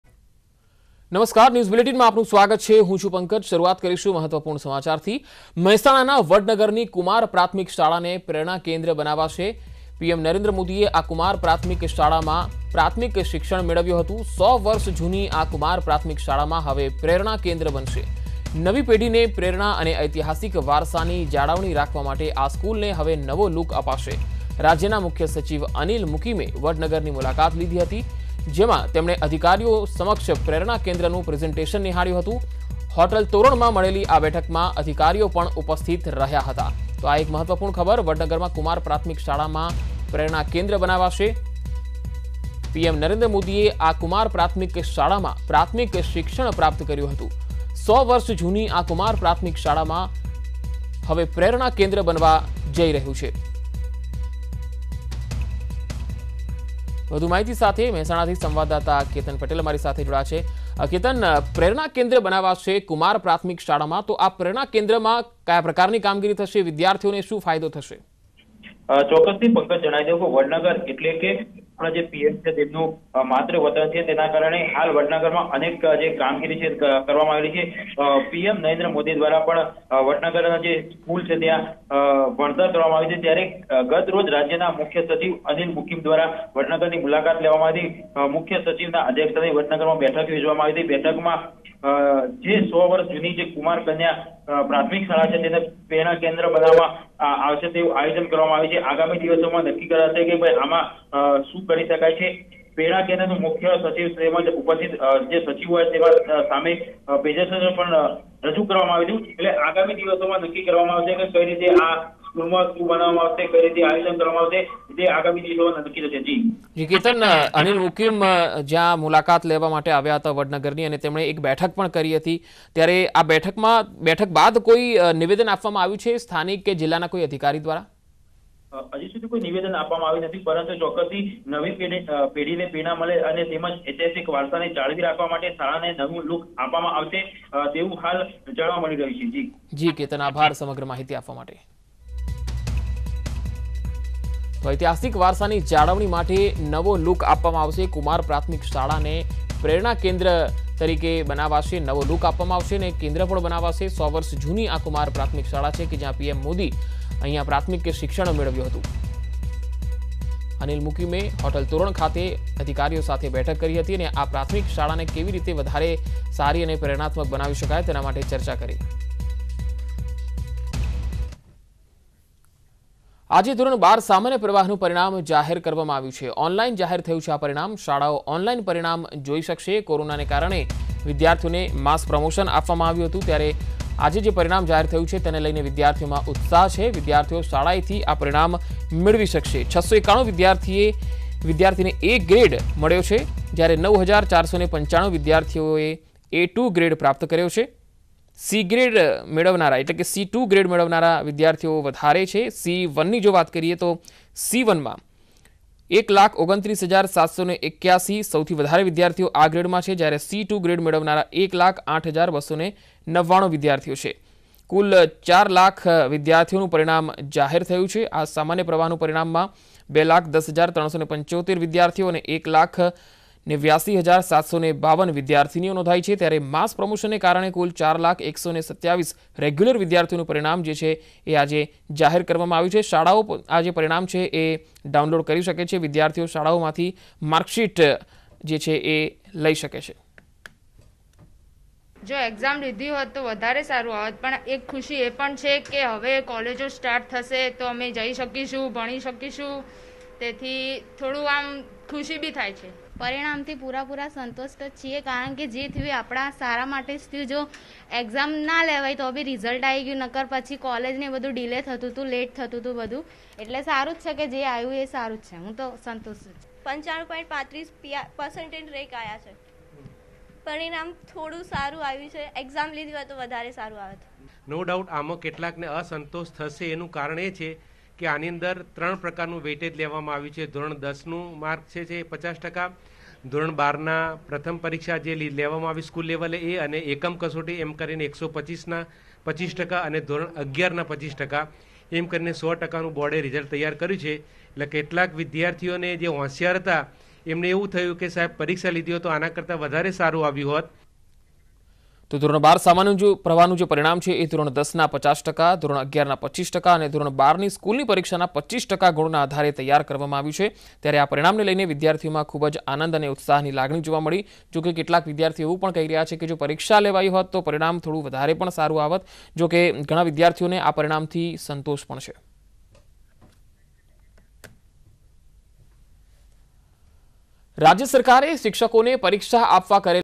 Mahesana | Vadnagar ની કુમાર પ્રાથમિક શાળાને પ્રેરણા કેન્દ્ર બનાવાશે | News18 Gujarati
Mahesana ના Vadnagar ની કુમાર પ્રાથમિક શાળાને પ્રેરણા કેન્દ્ર બનાવાશે.PM Narendra Modi એ મેળવ્યું છે આ શાળા માંથી પ્રાથમિક શિક્ષણ.100 વર્ષ જૂની કુમાર પ્રાથમિક શાળાને બનાવાશે પ્રેરણા કેન્દ્ર. નવી પેઢીને પ્રેરણા મળે તે હેતુ થી શાળાને નવો લૂક અપાશે